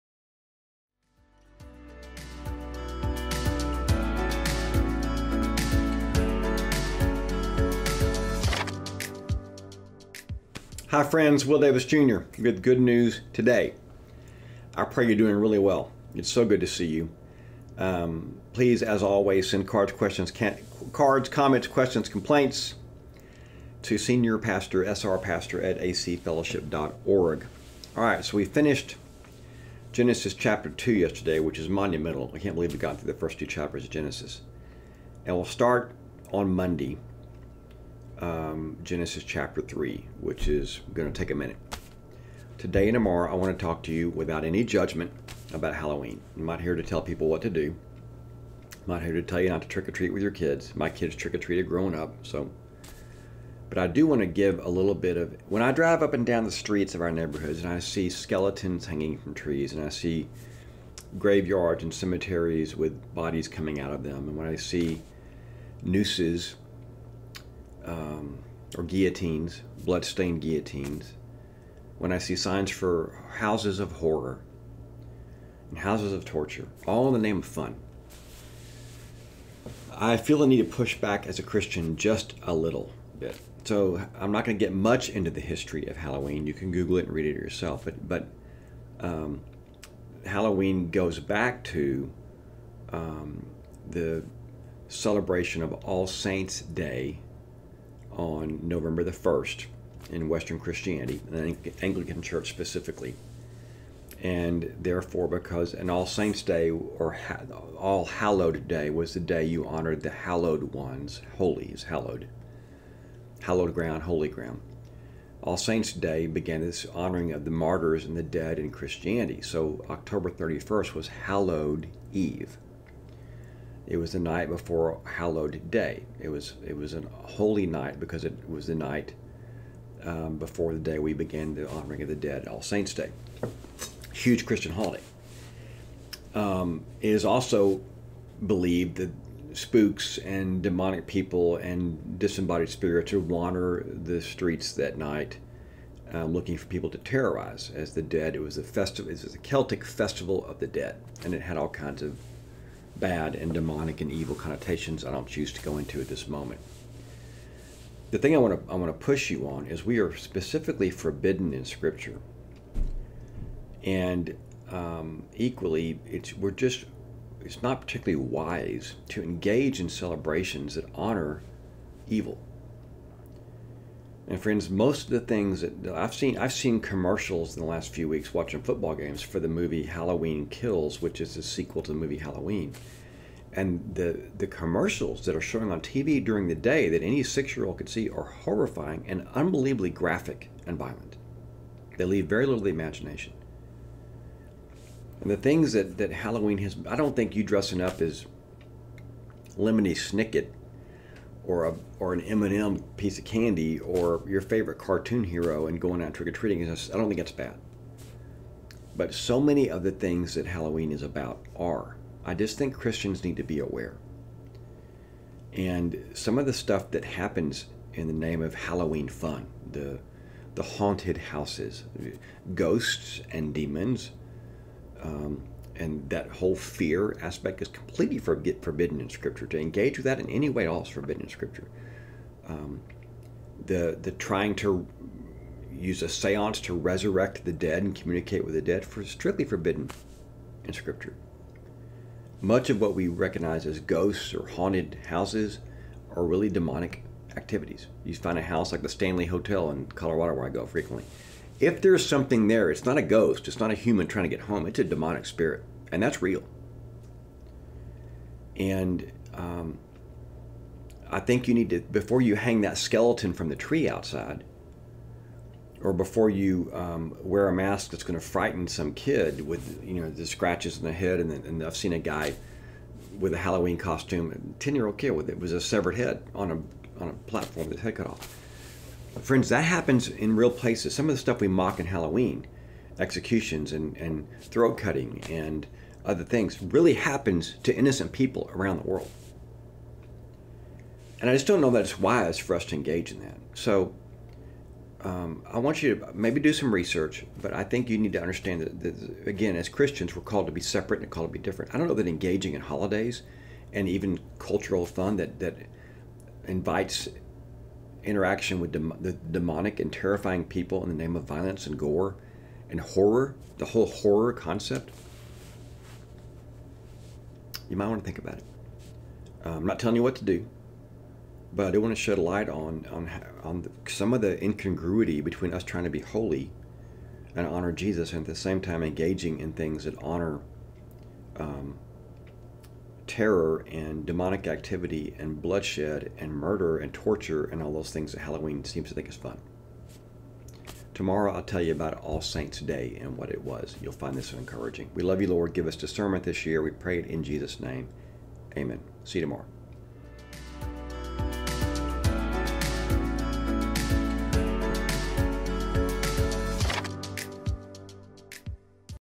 Good News Today | October 28
Halloween is just around the corner! What should we as Christ-followers make of it? Listen in as Pastor Will shares his thoughts on the good and not-so-good aspects of the holiday.